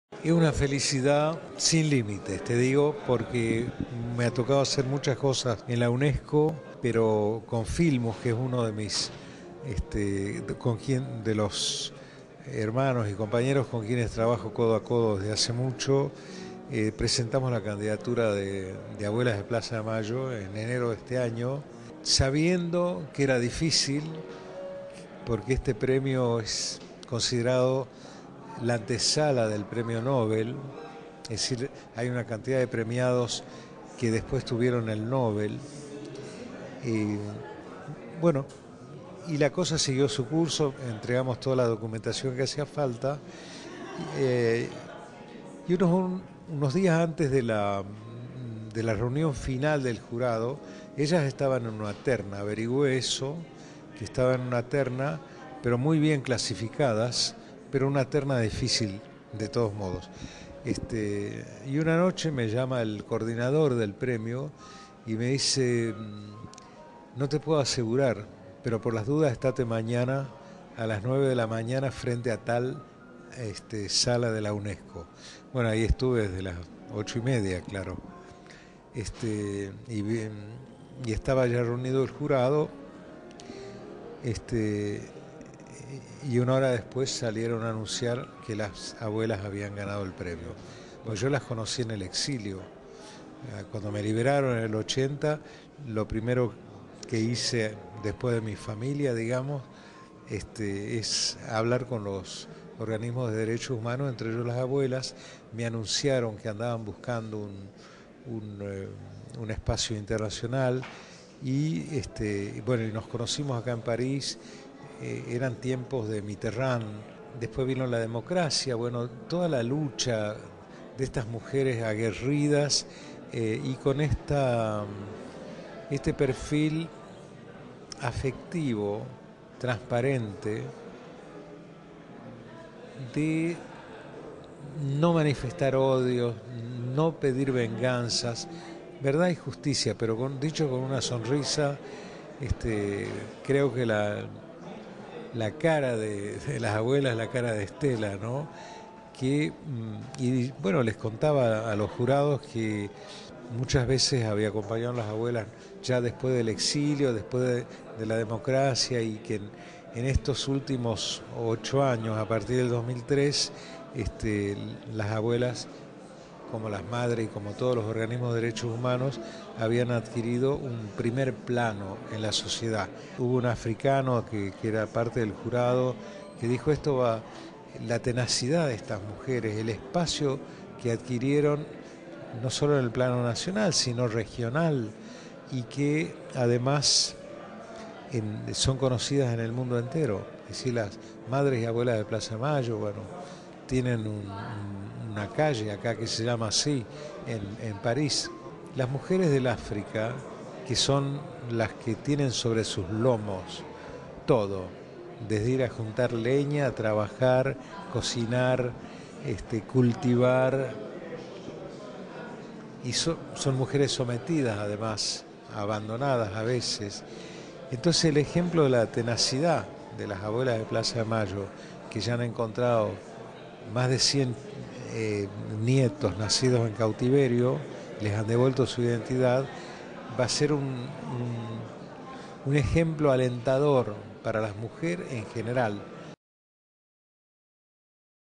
"Este premio es la antesala del Nobel"
Lo aseguró el embajador argentino ante la UNESCO, Miguel Angel Estrella minutos después de la entrega del premio Houpohuet Boigny a las Abuelas de Plaza de Mayo, en una ceremonia cálida y emotiva realizada en la sede de la institución en París.

"Cuando junto a Daniel Filmus presentamos la candidatura de las Abuelas en enero de este año, lo hicimos sabiendo que era muy difícil" afirmó Estrella en diálogo con la Agencia Télam.

"Pero hace pocos días me convocaron desde el jurado para hacerme un anuncio que finalmente fue la designación de las Abuelas para recibir este premio" agregó el pianista.

Estrella recordó cómo las conoció a comienzos de 1980, poco después de haber sido liberado por la dictadura militar y la forma en la que se fue estrechando su relación no solo con Abuelas sino con prácticamente todos los organismos de Derechos Humanos.

"Este premio constituye una reivindicación del papel de la mujer   por la tenacidad y por el mensaje de esperanzador y de paz que permanentemente invocan, además de la tarea impresionante que permitió la devolución de sus identidades a más de un centenar de nietos secuestrados por la dictadura" afirmó Estrella.